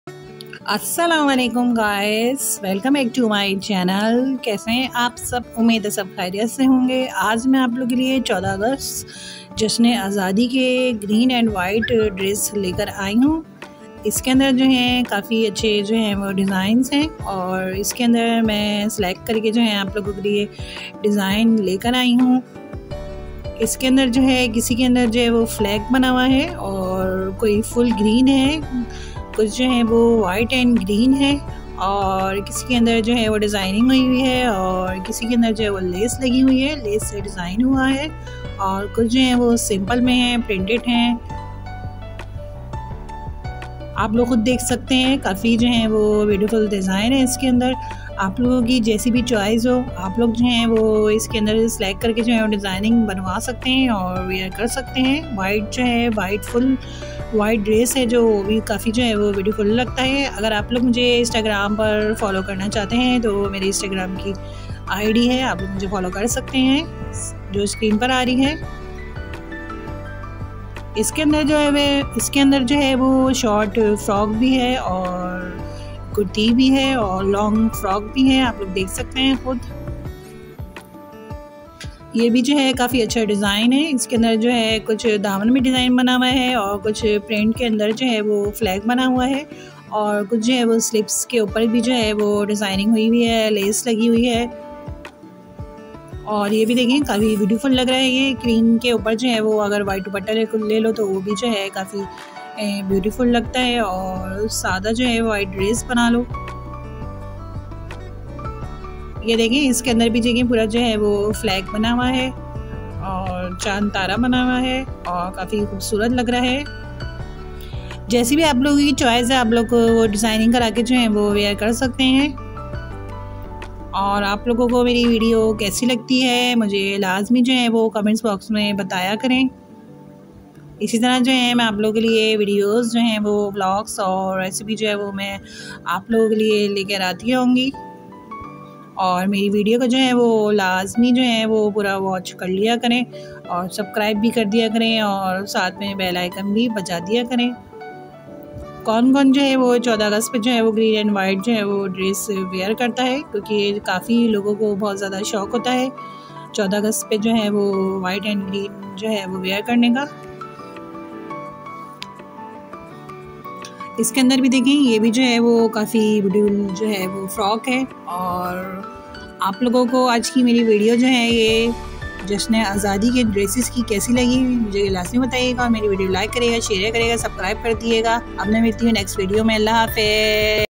14 August Independence Day Dress Designs Ideas 2021 | With Saabi World Recipes#
14 August Independence Day Dress Designs Ideas 2021 | With Saabi World Recipes# Happy Independence Day log bht Josh o Jazbe se manate hen aur Girls green & White dress b wear krti hen